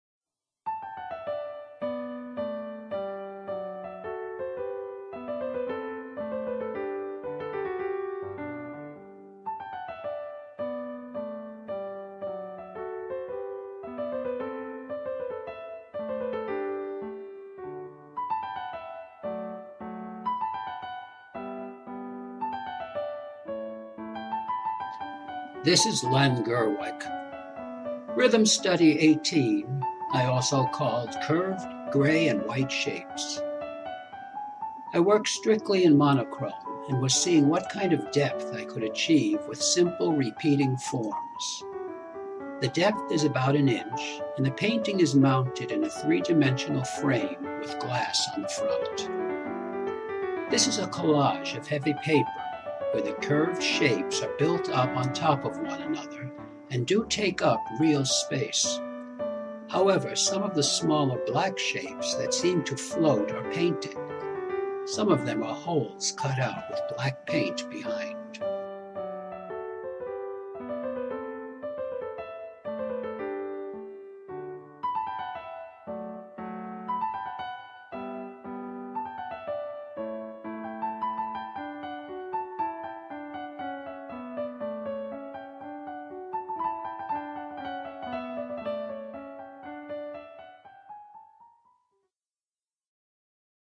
Collage Art Abstract Acrylic Painting 3D Shapes- MA18 Rhythm Study
Three dimensional collage of monochromatic shapes that create a sense of rhythm and movement.  See pricing at gerwickpaintings.com